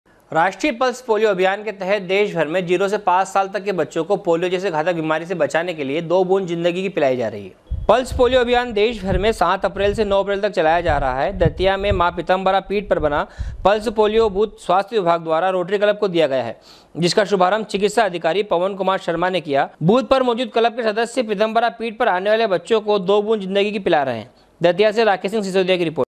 SW 24 News 07 04 2019 Datiya
राष्ट्रीय पल्स पोलियो अभियान के तहत देषभर में 0 से 5 संाल तक के बच्चो को पोलियो दवा पिलाई जा रही है ।